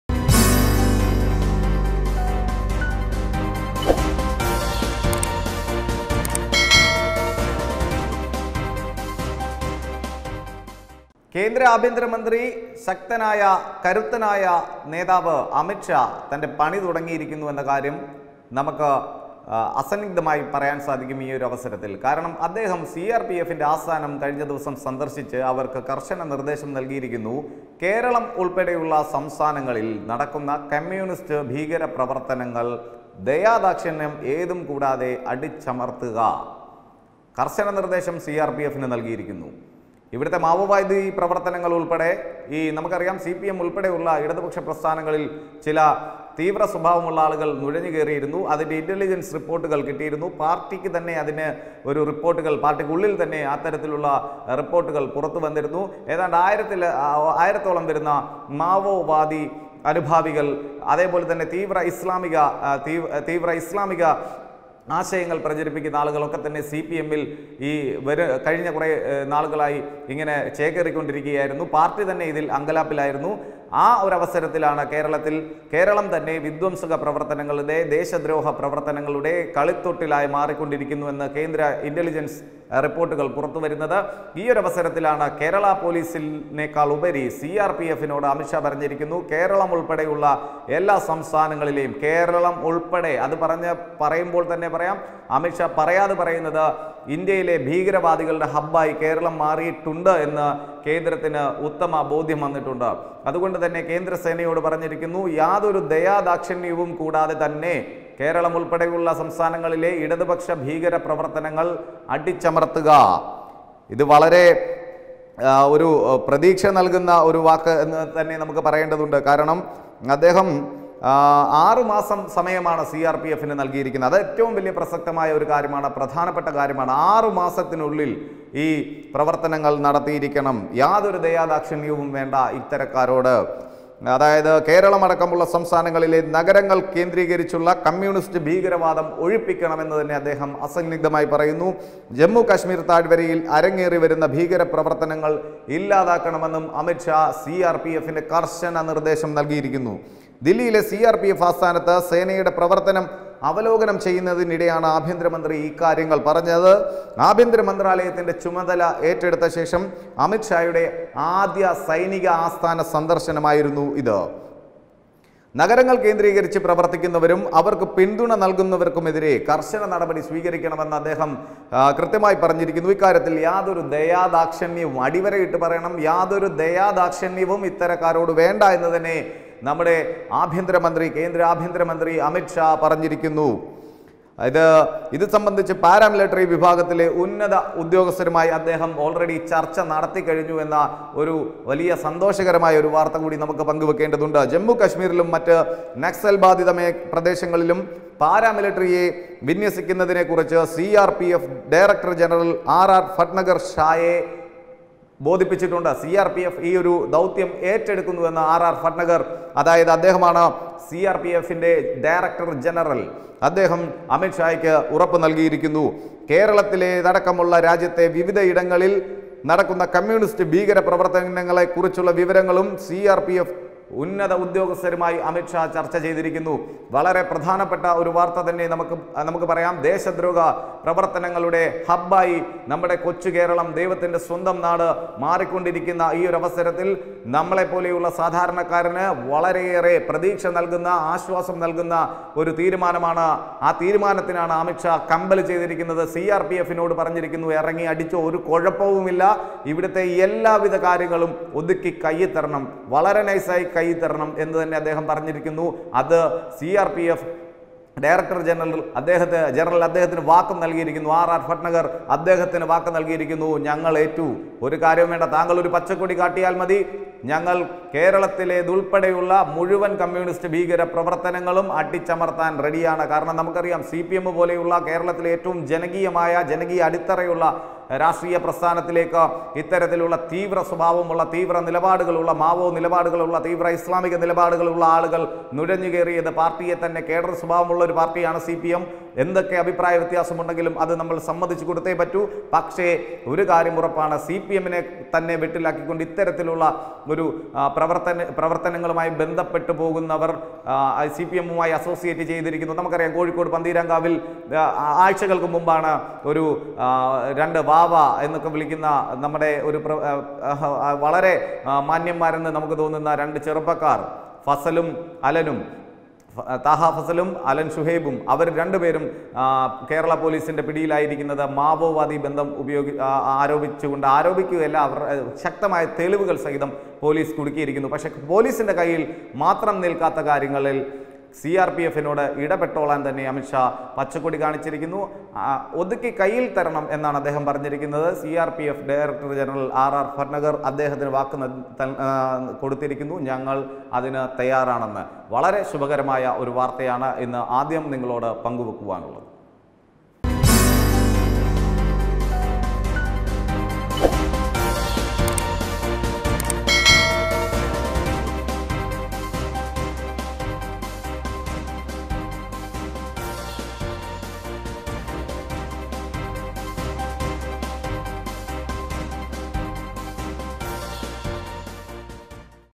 അമിത്ഷാ കട്ടായം പറഞ്ഞു...കമ്മ്യൂണിസ്റ്റ് തീവ്രവാദം തുടച്ചുമാറ്റാൻ സി.ആർ.പി.എഫ് രംഗത്ത്...
കേരളത്തിലുൾപ്പെടെ എല്ലാ സംസ്ഥാനങ്ങളിൽ നിന്നും കമ്മ്യൂണിസ്റ്റ് തീവ്രവാദം ഉൾപ്പെടെയുള്ള എല്ലാ തീവ്രവാദങ്ങളും അമർച്ച ചെയ്യാൻ സി.ആർ.പി.എഫിന് നിർദേശം...